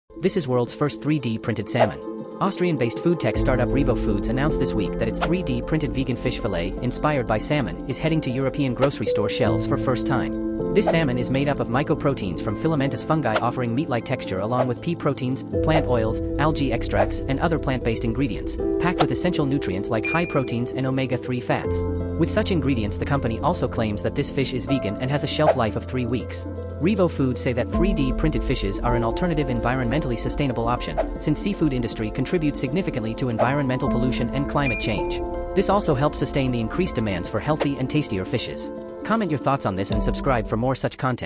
Fish meat but not a fish!!!!
Most of you love salmon, but with the marine pollution rising many question the potential toxic and health benefits of that fish. however, thanks to the Revo food startup company, we now have salmon meat completely 3D printed using plant and fungi based raw ingredients which is packed with all the essential nutrients necessary. It is the future of food industry, all thanks to science and technology, says the Austrian startup